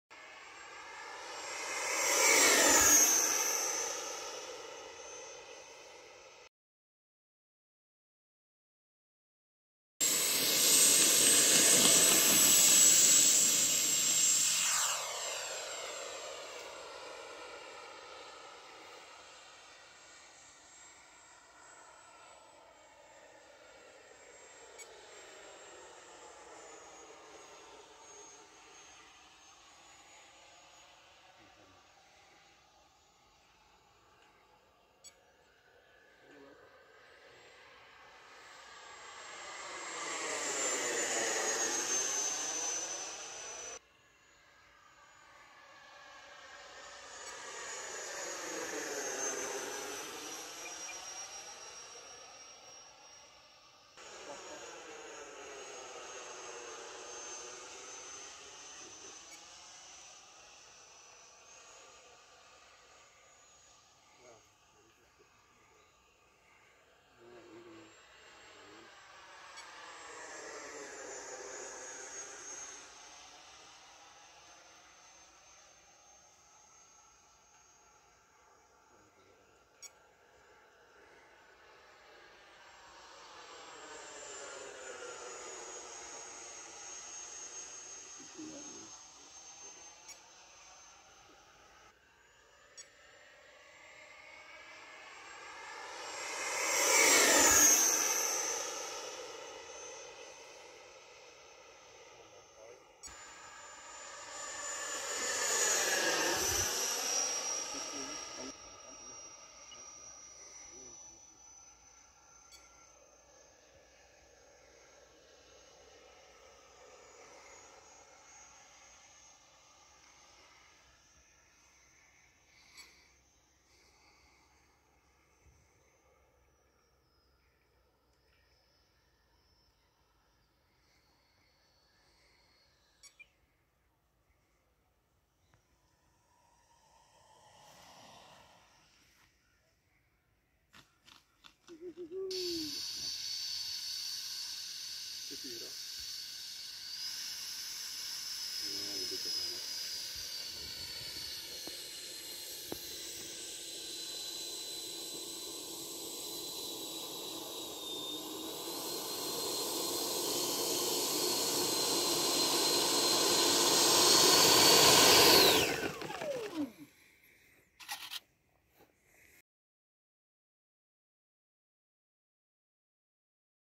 E-Flite F-16 Falcon 80 mm EDF, first flight on grass runway
First flight of these wonderful E-Flite F-16 Falcon 80 mm EDF on our local grass runway.
Not my machine, but I was asked to take her to the sky for the first flight and check everything.
It was an easy ride, testing flaps, retracts, checking CG etc.
A little flaperon was used to take of from the grass runway, full flaperon during the landing. 6S 5000 mAh, we've set the timer to 2:30 which allows for a go-around in case of a non-ideal landing approach.

I'm sure this airframe has a lot more to offer than shown here, but again, always easy on the first flights.